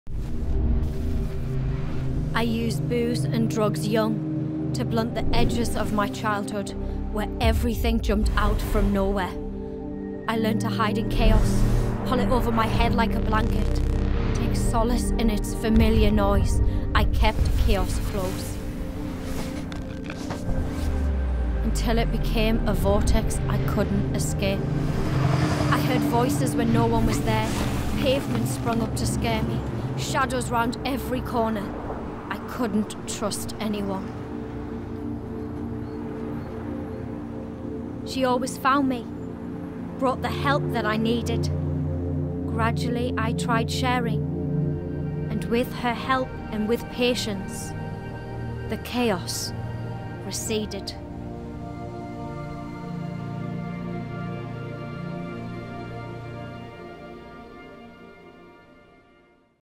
"With her help, the chaos receded" | CHOOSE PSYCHIATRY
A film about working as a psychiatrist specialising in supporting homeless people, part of the RCPsych Choose Psychiatry series.

Find out more about a career in psychiatry: rcpsych.ac.uk/choose